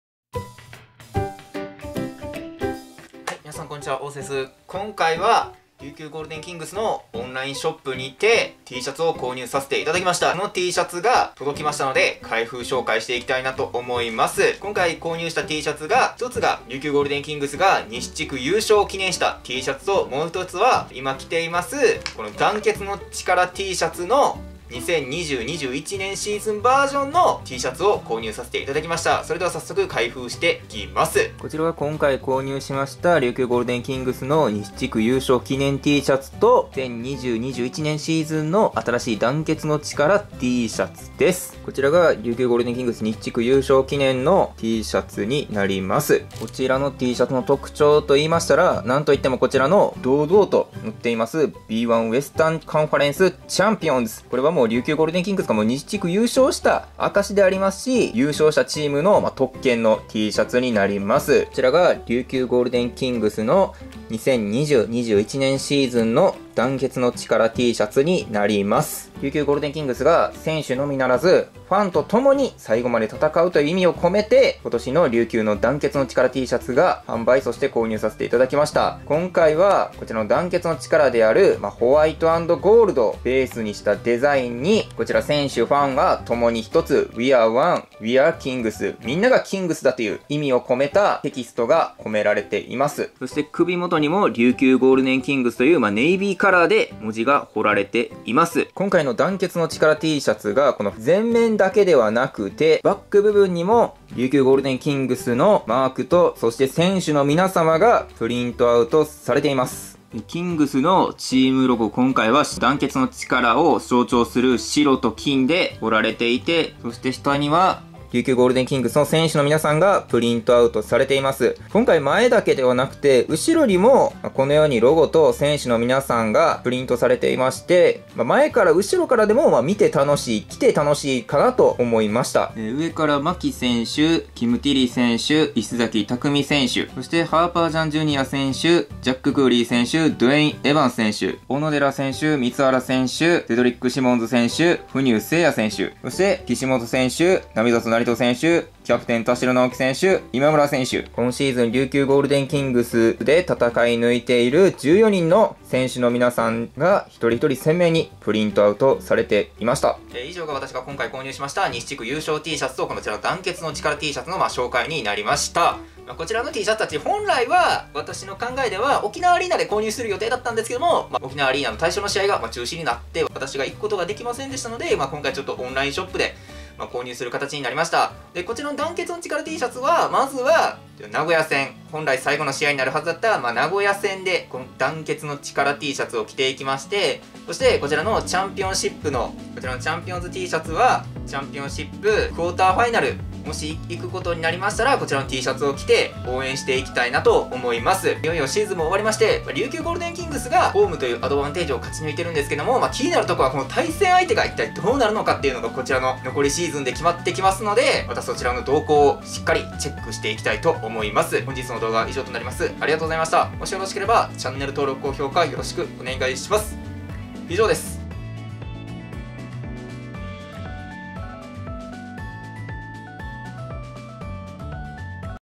琉球ゴールデンキングス 西地区優勝Tシャツ 団結の力Tシャツ購入
琉球ゴールデンキングスのTシャツを購入しました。
このTシャツを着て最後まで琉球ゴールデンキングスを応援します。